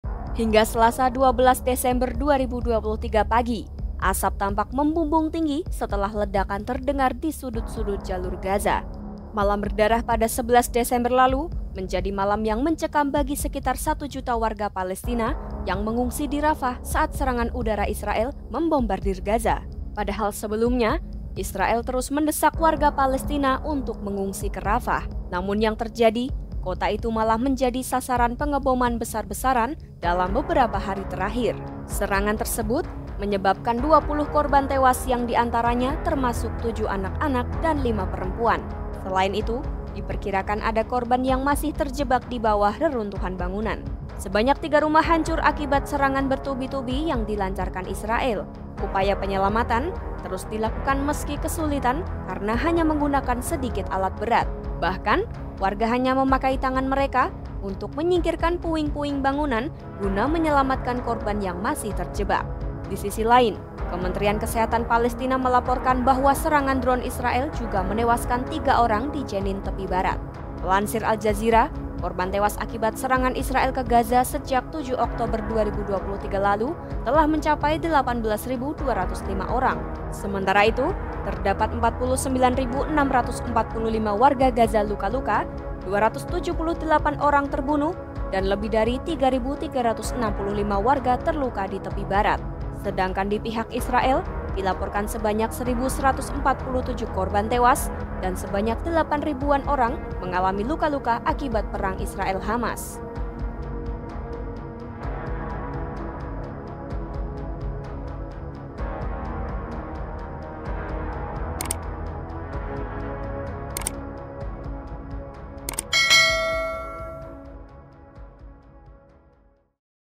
Serangan Berdarah Israel di Jalur Gaza Tewaskan 20 Orang di Rafah
Hingga Selasa (12/12/2023) pagi, asap terlihat membumbung tinggi setelah ledakan terdengar di Jalur Gaza. Malam berdarah pun dialami oleh satu juta warga Palestina yang terpaksa mengungsi ke Rafah, kota selatan Gaza saat serangan udara Israel digencarkan.

Serangan Israel tersebut pun menyebabkan 20 orang tewas, di antaranya termasuk 7 anak-anak dan setidaknya 5 wanita. Selain itu, terdapat korban lainnya yang diperkirakan masih terjebak di reruntuhan bangunan.

Upaya penyelamatan pun terus dilakukan meski kesulitan karena hanya menggunakan sedikit alat berat. Bahkan warga hanya menggunakan tangan mereka untuk menyingkirkan puing-puing bangunan untuk menyelamatkan korban yang terjebak di bawahnya.

Penulis Naskah: Adinda Septia Berliana
Narator: Adinda Septia Berliana
Video Editor: Adinda Septia Berliana
Produser: Holy Kartika Nurwigati Sumartiningtyas